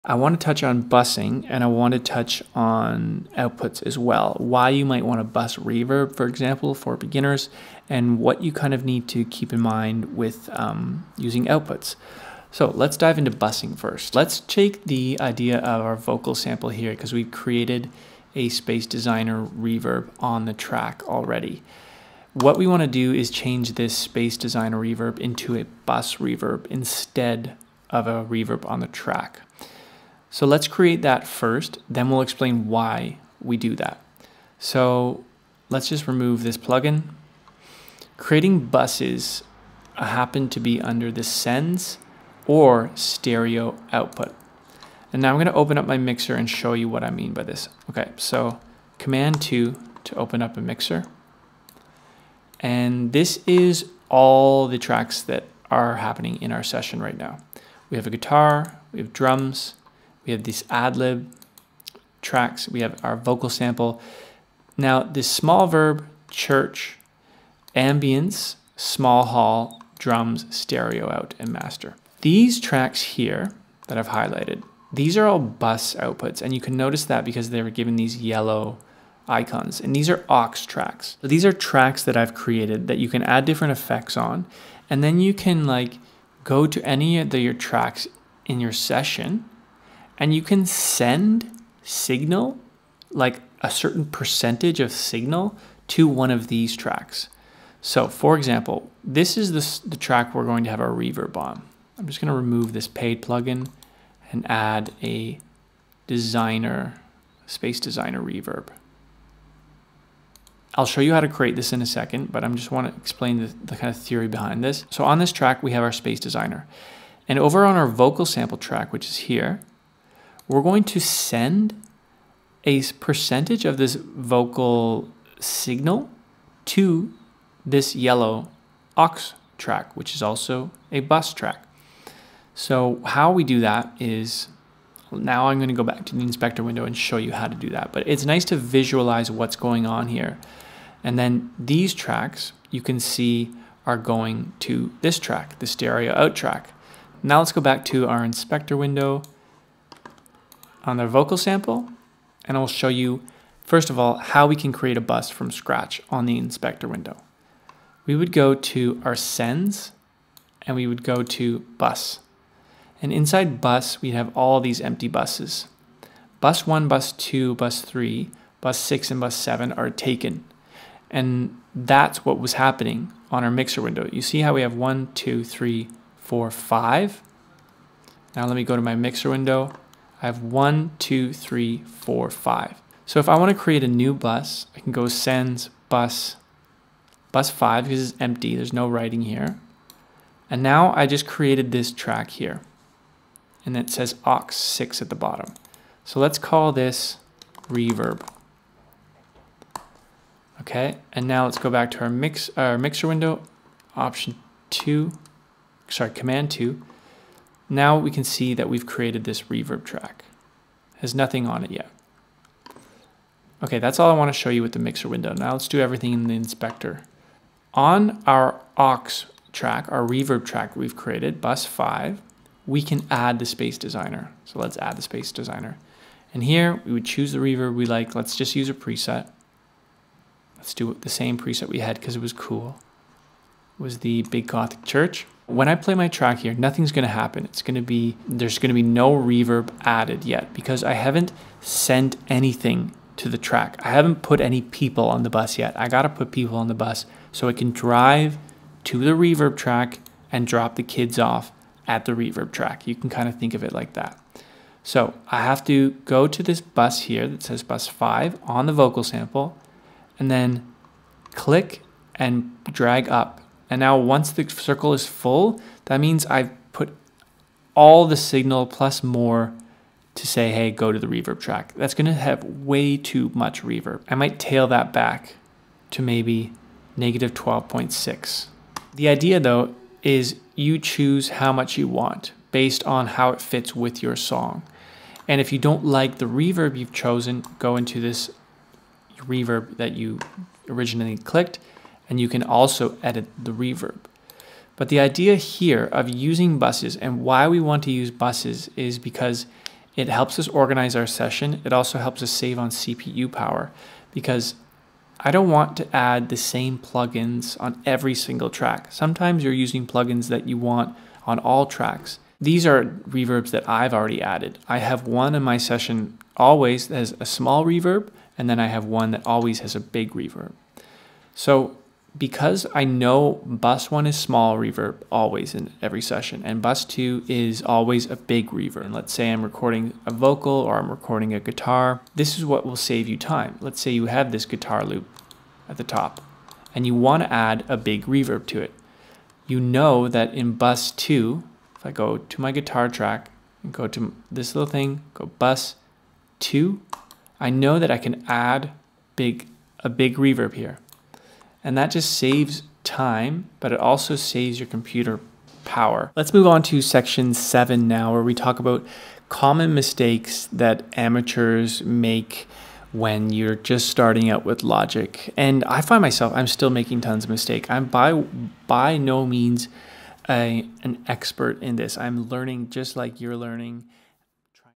How To Do Busing in Logic Pro X
If you're making music then you need to know how to do busing in logic pro x. Learning how to bus in logic or any DAW for that matter will not only improve your productivity but your computer will be very happy about it. I typically will have many bus outputs saved in templates so I can quickly add reverb or delay right on the channel. If you've never heard of busing that's okay too, I will break it down from scratch and show you how you can get started. A typical charles cleyn logic pro x tutorial where we move at a beginners pace with the hope of making you a busing expert in logic at the end of the video. Let me know if have any questions on how to do busing, music production, music marketing, or logic in a comment. I'd be happy to help.

Release music?